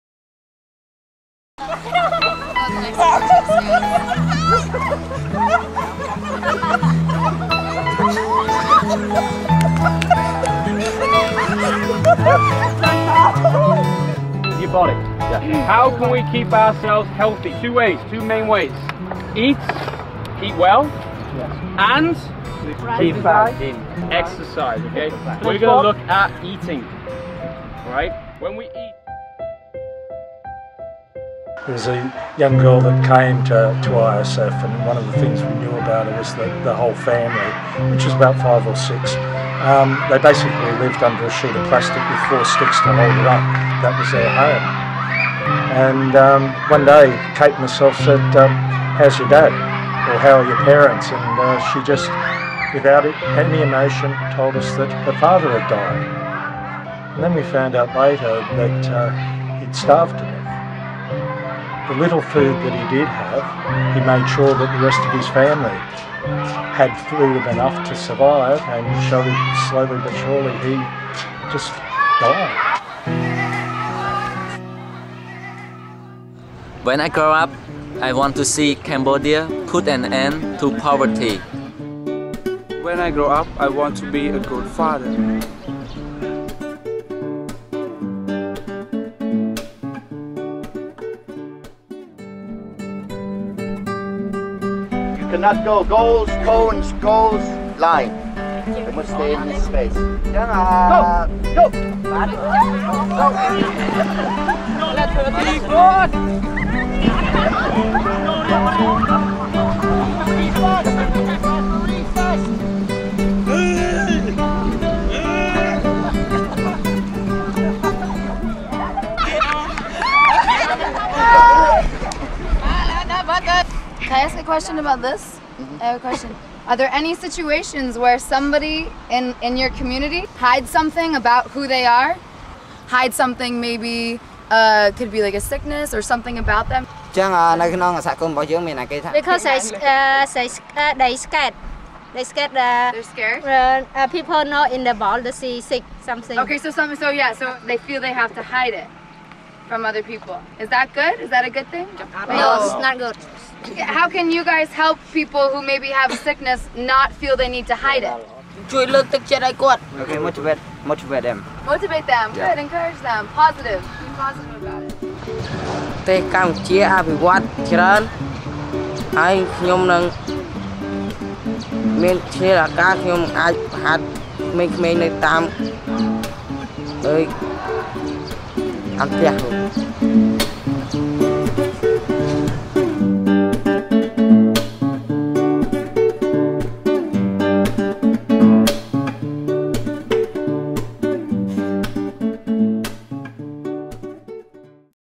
Coaches Across Continents Football for Health and Wellness 2015
This video shows the partnership between Coaches Across Continents and ISF, Cambodia.